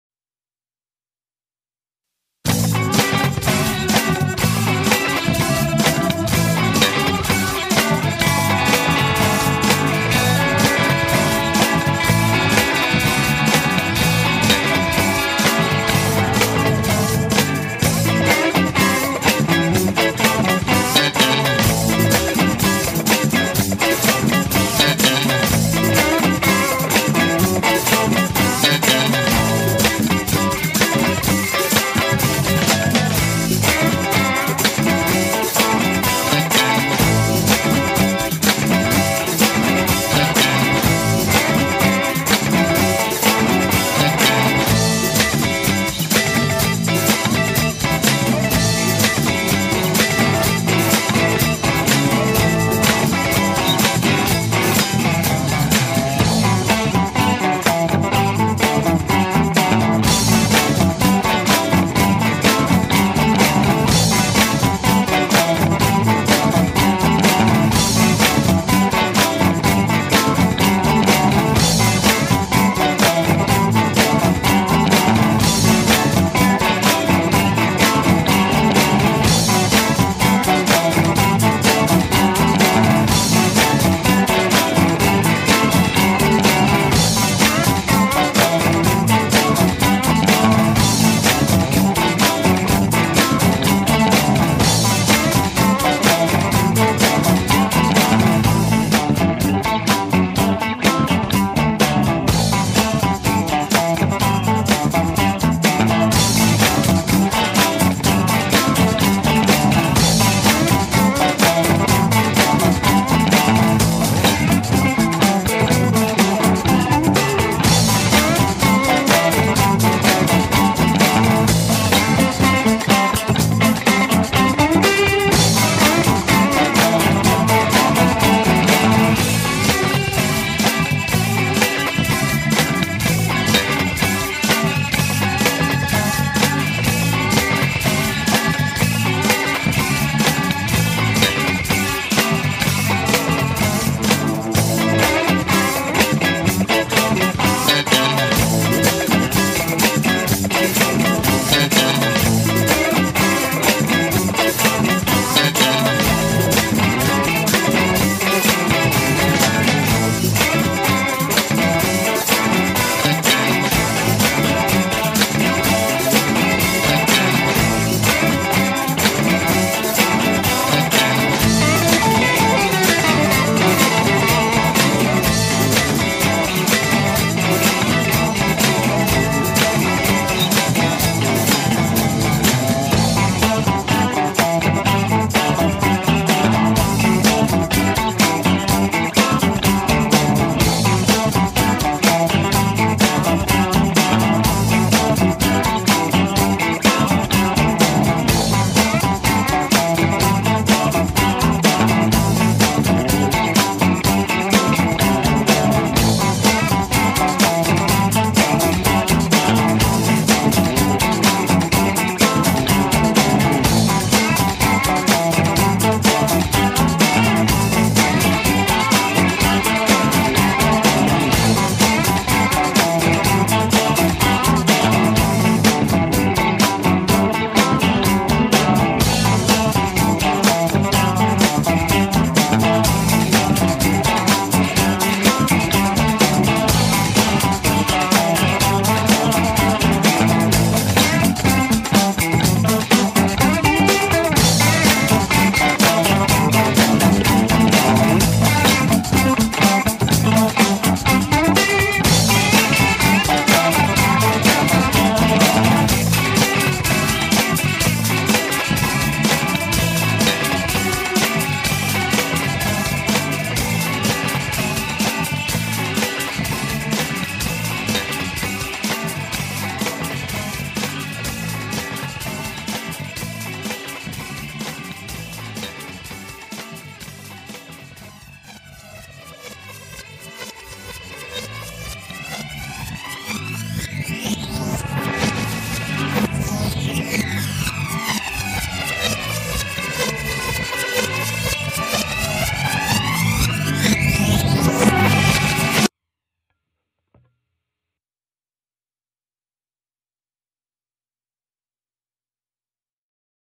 Guitar Faces by Scott and Pamela Baxendale
These are Guitar Faces Pamela makes from the scraps of the guitars I build. The music is a tune I did a few years ago with my Bad Bax Band with Lewis Quinn on drums and Colin Mitchell on Bass.